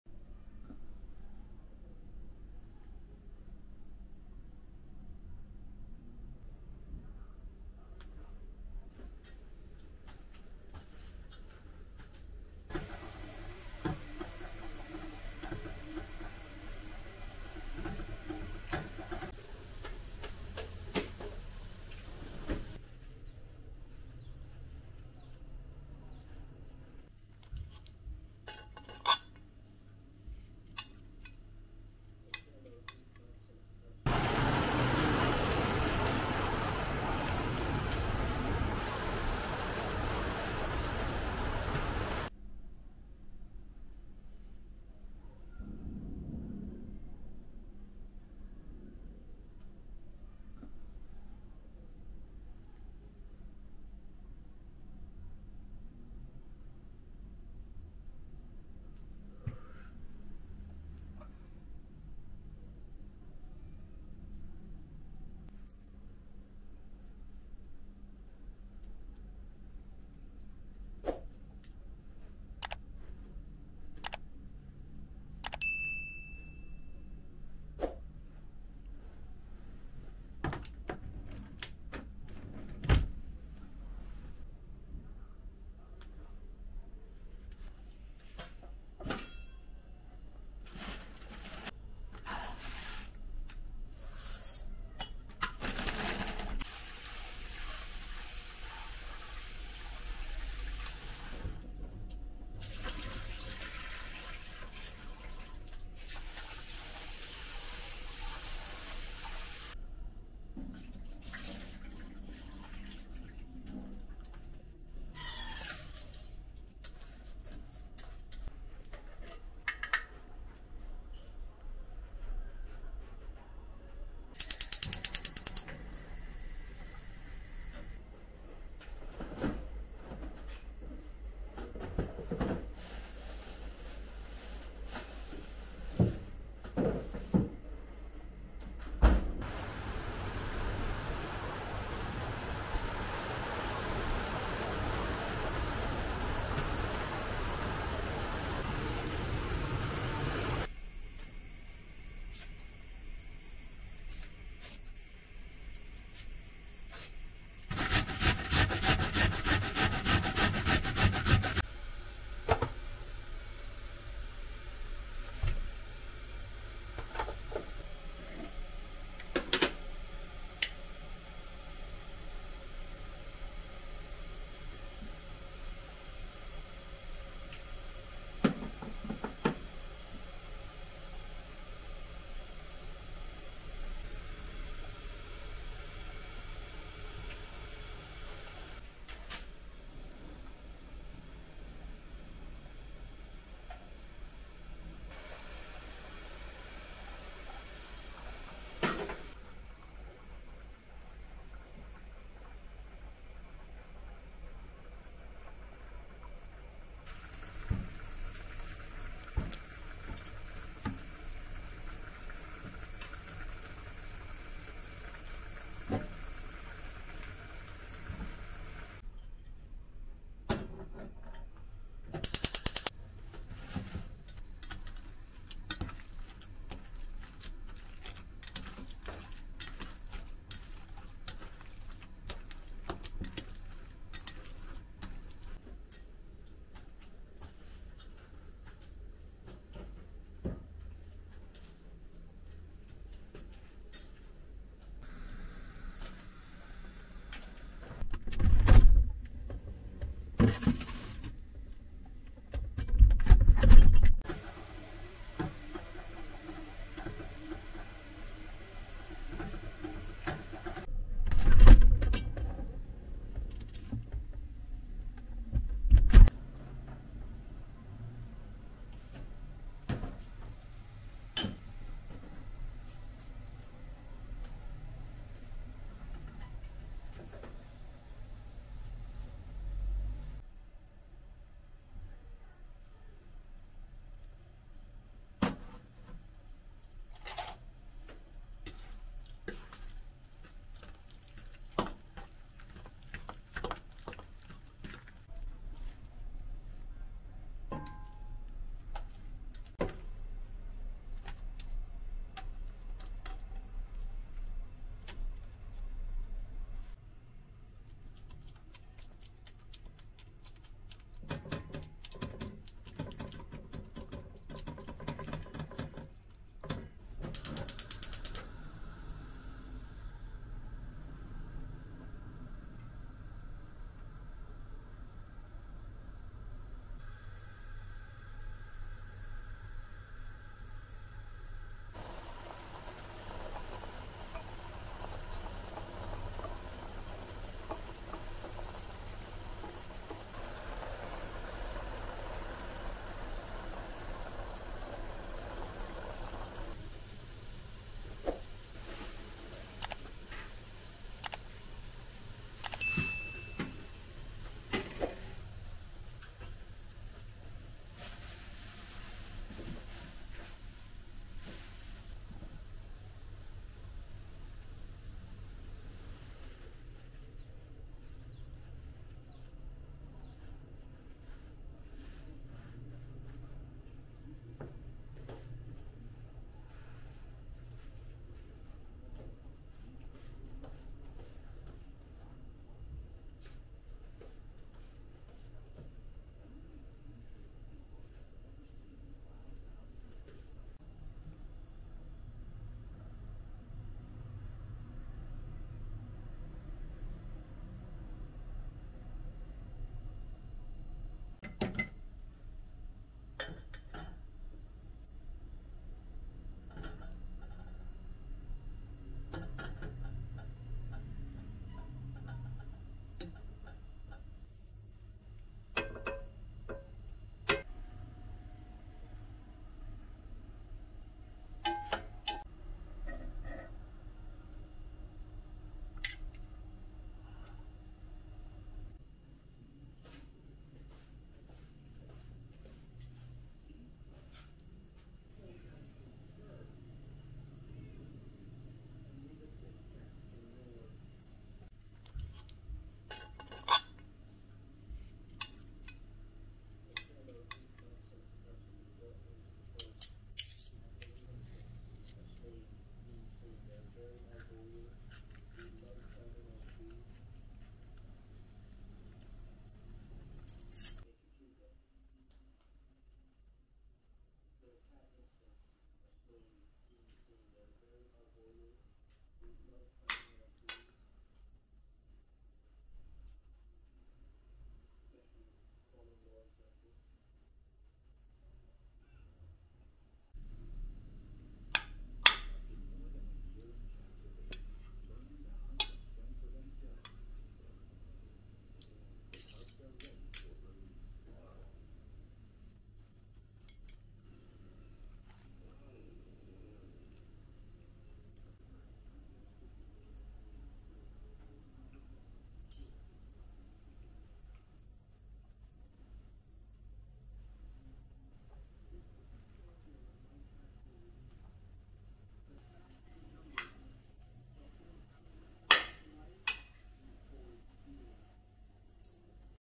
Chicken kebab, new recipe with a new method by Kandsevgisi
How to prepare chicken kebab with the new freezing method by me🙊